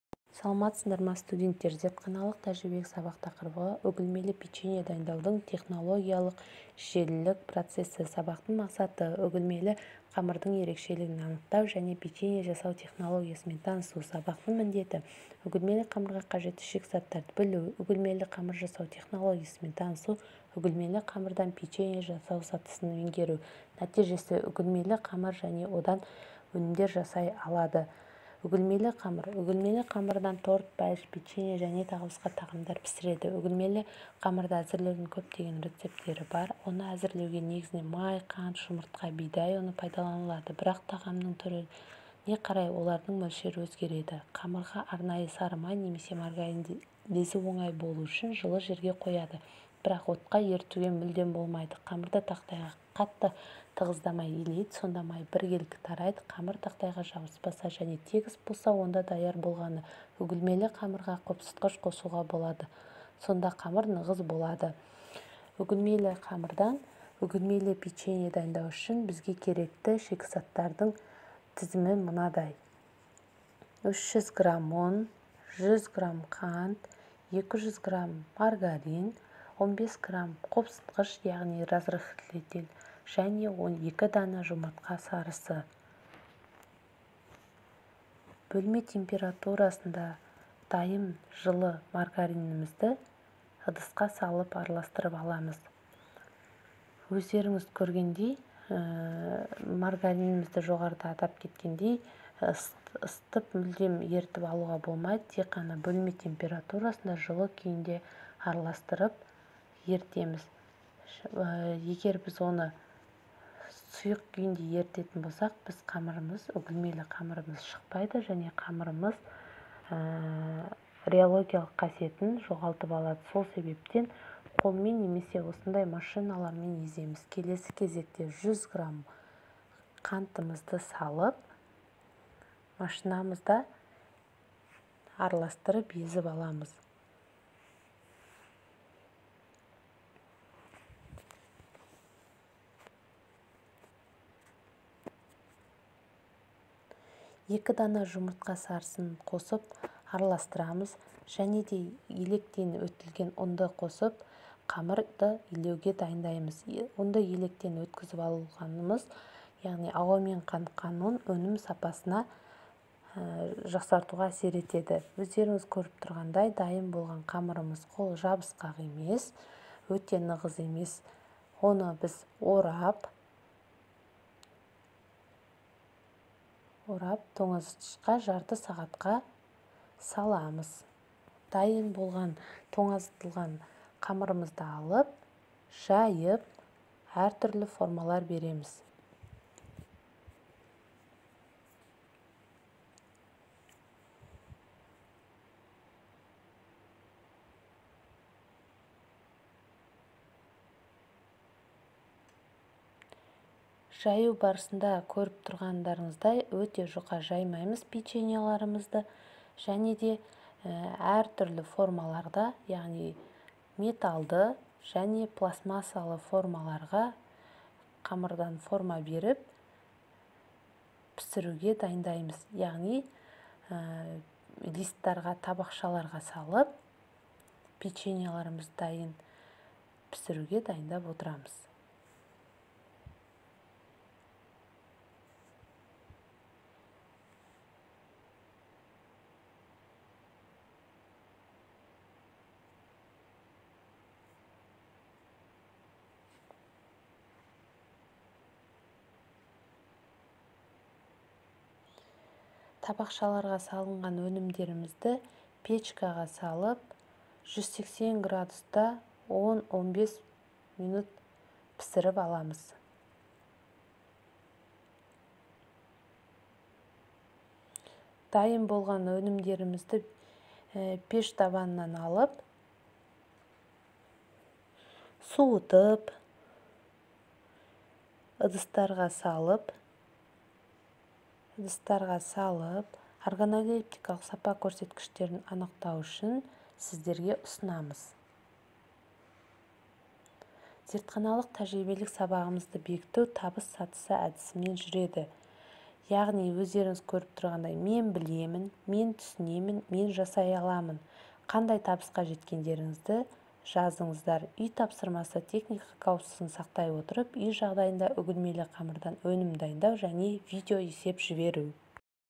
Үгілмелі қамырдан печенье жасаудың технологиялық желілік процесі. Муратова М.М.
КМ 01.